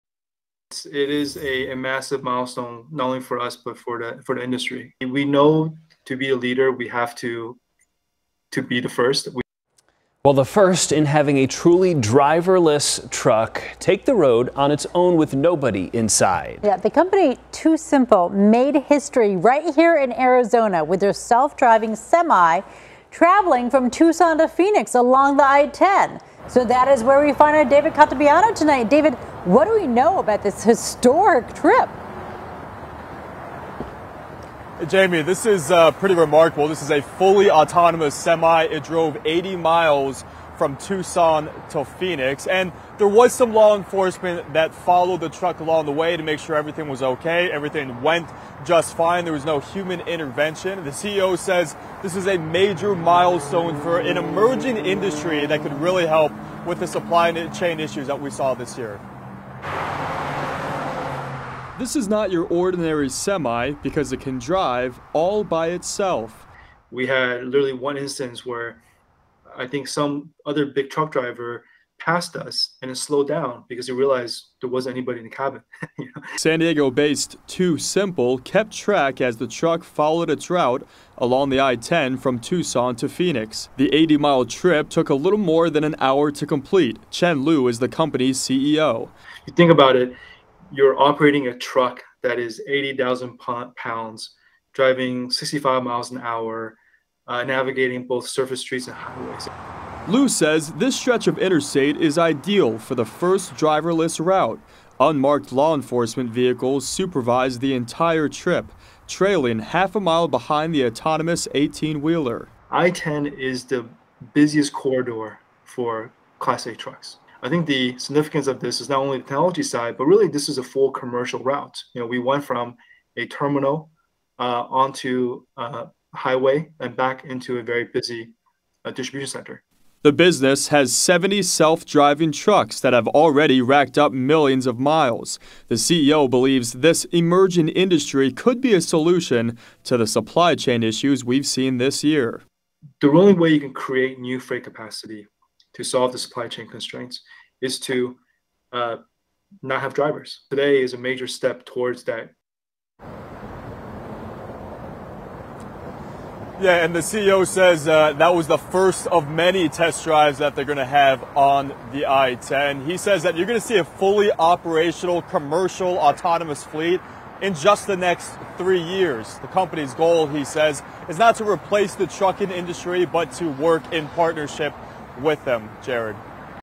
Autonomous truck company completes first no-human road test
San Diego-based TuSimple says it’s the first successful fully-autonomous run by a class 8 vehicle, or semi, on open public roads with no human intervention.